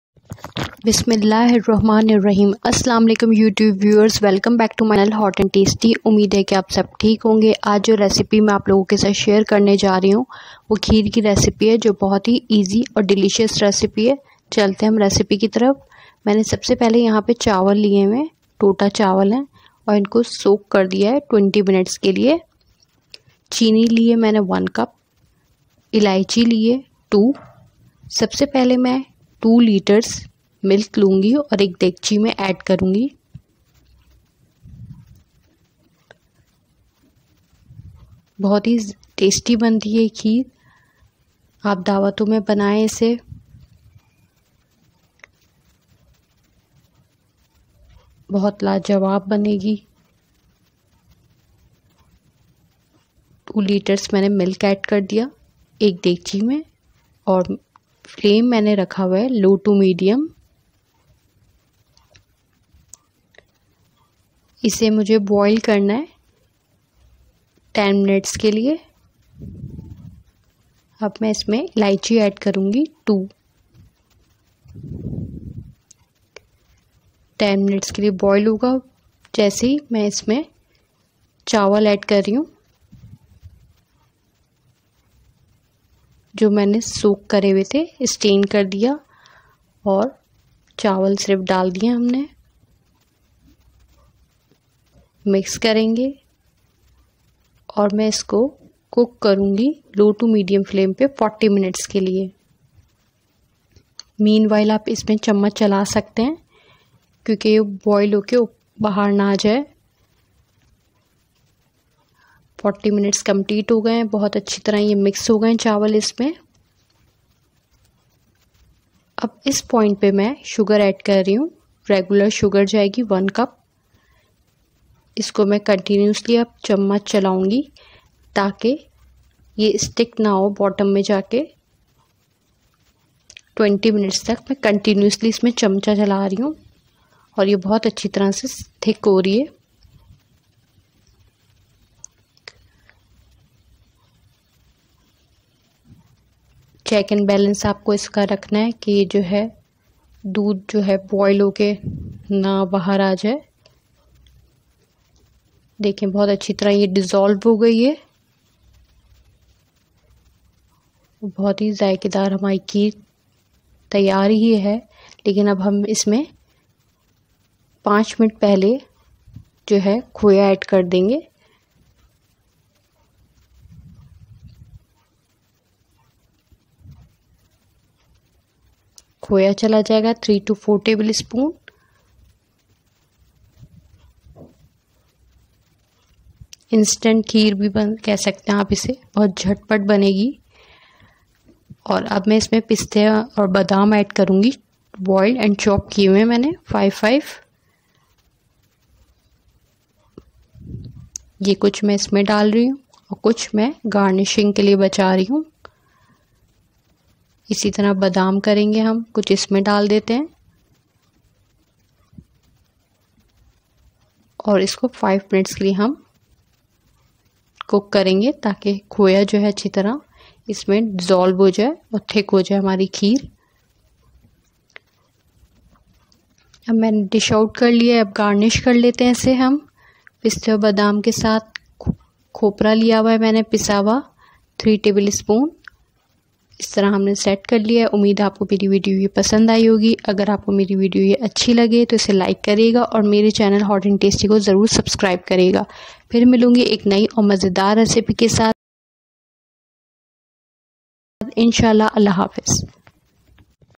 Kheer Recipe | Rice Kheer | Chawal Ki Kheer | Sweet Dish Recipe By Hot & Tasty
Learn how to make easy & delicious Kheer. A quick and easy dessert recipe with scrumptious taste.